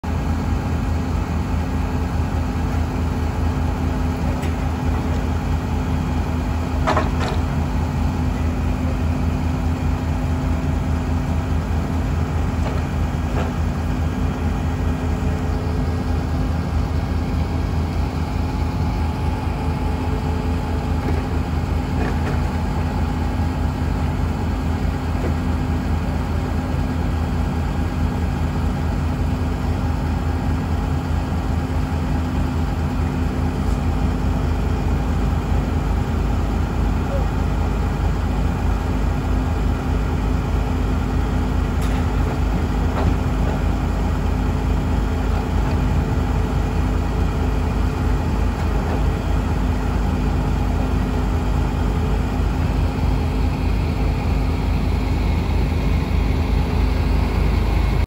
2011 Ponsse Wisent 6 Wheeled Forwarder
2011 Ponsse Wisent 6 Wheeled Forwarder. Hour meter reads 15,581 hours. Mercedes engine. 700/55-34 tires on the front are estimated at 75%. 710/45-26.5 tires on the rear are estimated at 80%. Can include double diamond chains at 90% and Clark tracks at 60% for an additional $5,000. K70+S crane is tight. Center was just re-pinned and is tight. New bucket installed in 2021. New clutch plate. Heated and leveling seat. Webasto preheat system. Heat, A/C, radio, lights.